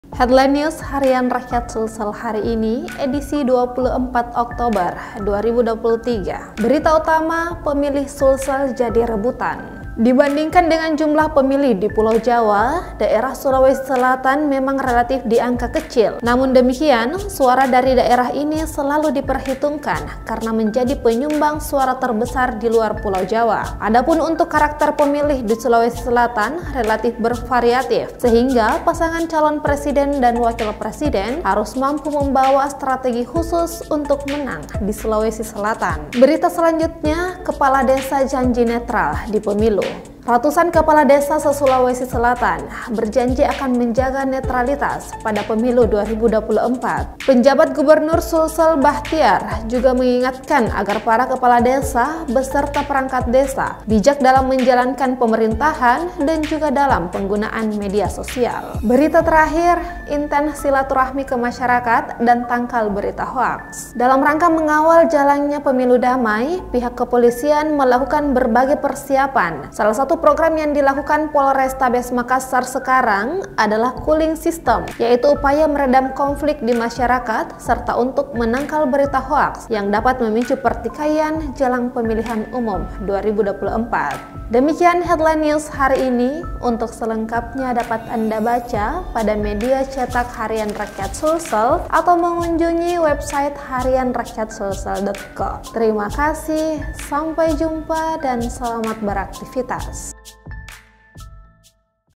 Pilpres 2024: Pemilih Sulsel Jadi Rebutan | Headline News
Headline News Rakyat Sulsel 24 Oktober 2023
1. Pemilih Sulsel Jadi Rebutan
2. Kepala Desa Janji Netral di Pemilu
3. Intens Silaturahmi ke Masyarakat dan Tangkal Berita Hoaks